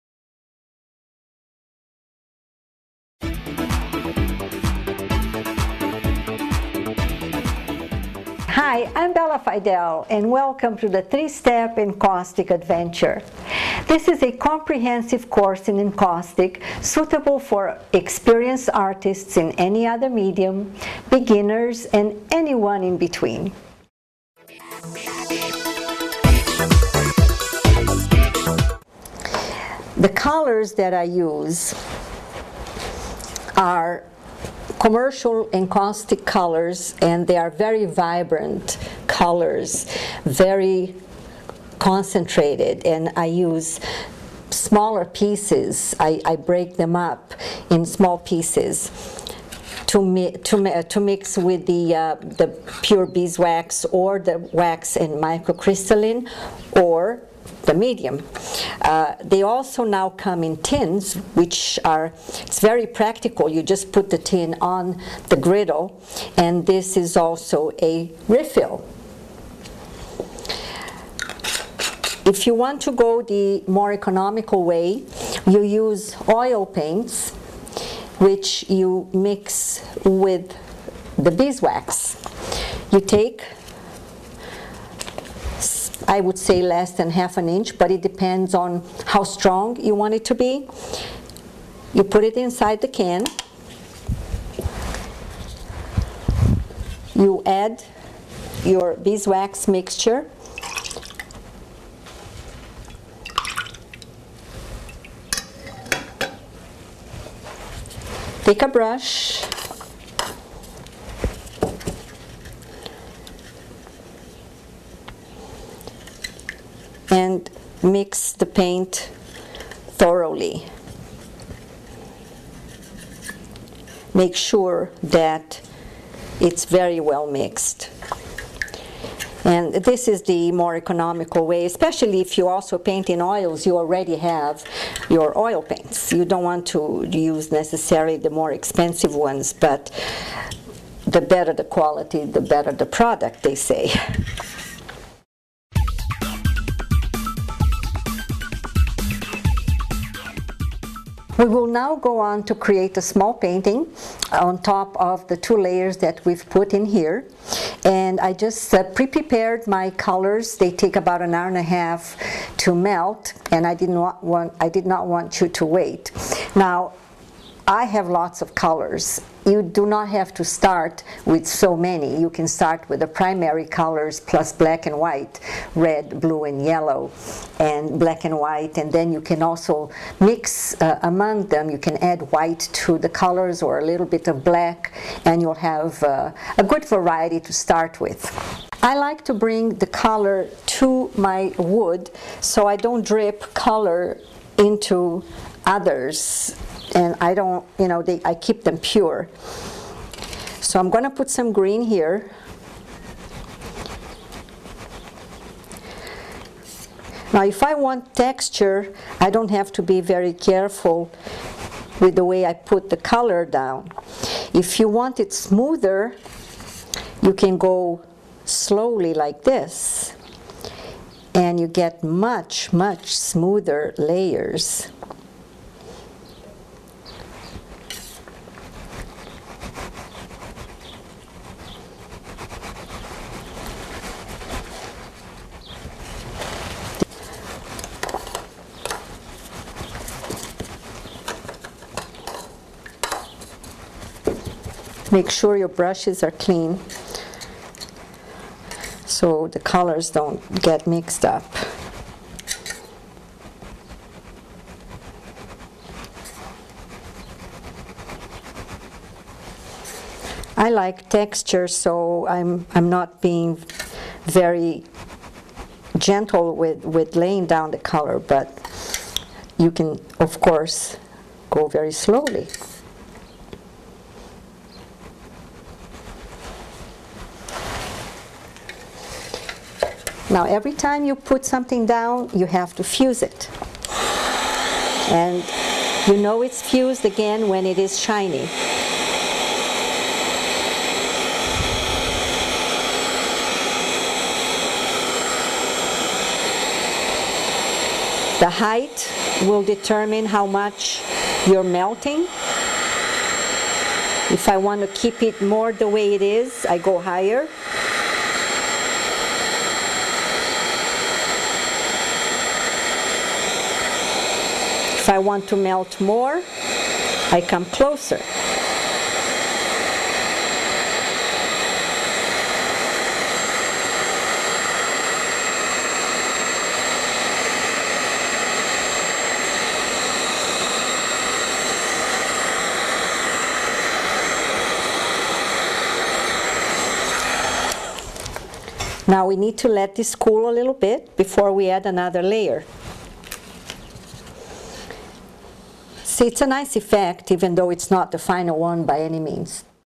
Preview | Encaustic Adventure with Bela Fidel, Parts 1 and 2: Preparation & Layering
Take the first steps into your Encaustic Adventure with Bela Fidel as she brings you tips for creating rich, dimensional works in wax After a review of supplies and safety (especially important when working with hot materials), Bela shows you how to mix your own vibrant colors and layer them for beautiful results. Once you have the techniques down, Bela takes you through the steps to create an abstract landscape using layers of color and a variety of application techniques.

Preview Encaustic Adventure (Parts 1 and 2): Preparation & Layering Now!

Born and raised in Brazil, Bela found her art niche after moving to Israel, and refined her technique with many years of academic training in her homeland and in the U.S. Currently teaching workshops in oil and encaustic in the Arizona area, she brings her techniques for working with luscious layers of wax to you now